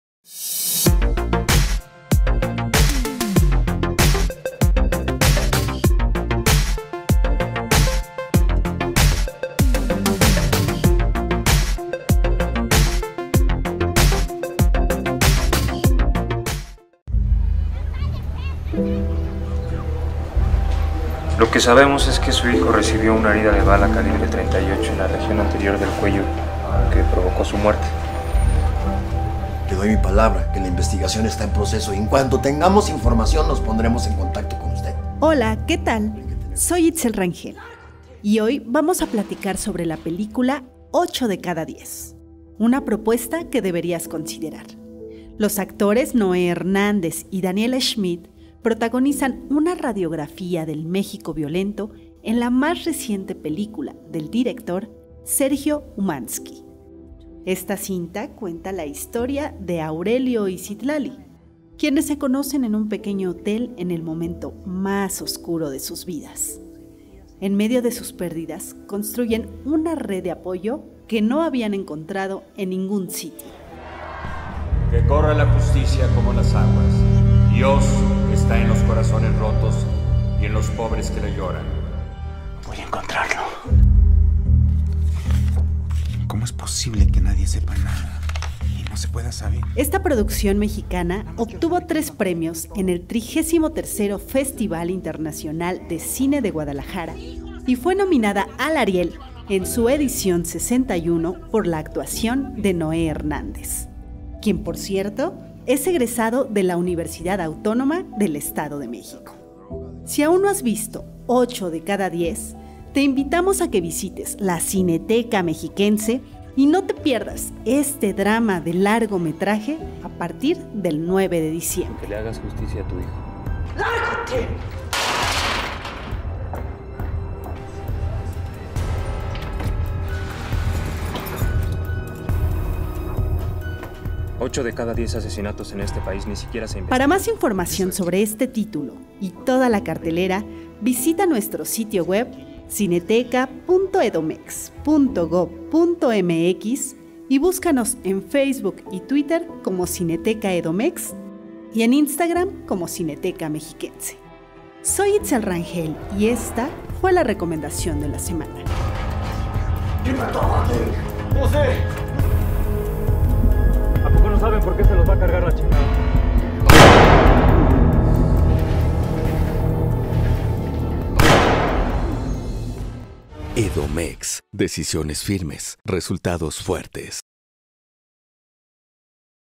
HablemosDeCine. "Ocho de cada diez".
Este jueves en HablemosDeCine 🍿te recomendamos el filme📽️ "Ocho de cada diez" cinta del realizador Sergio Umansky que ofrece una historia de denuncia y donde el amor es la única forma de esperanza para soportar la terrible realidad. No te la pierdas en ⏯️CulturaEnUnClick🎬